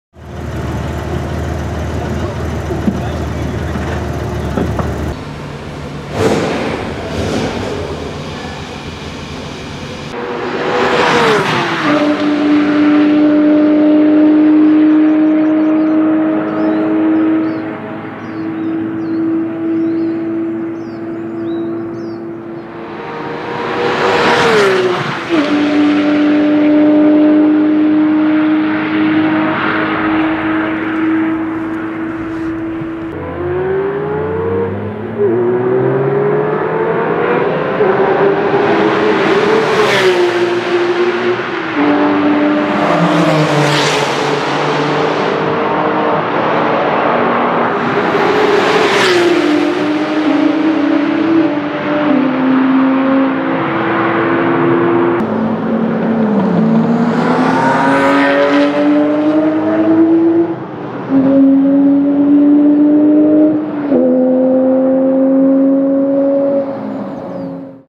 Porsche 997 GT3 RS 4.0 Fast Fly By's! LOUD Sound!
WATCH THIS VIDEO IN 1080P HD FOR GOOD QUALITY!

I have filmed some Porsche 997 GT3 RS 4.0 during a track event on Spa Francorchamps (Formula 1 circuit) You hear some great engine sounds with the fast fly-by's! Enjoy!!

CvdZijden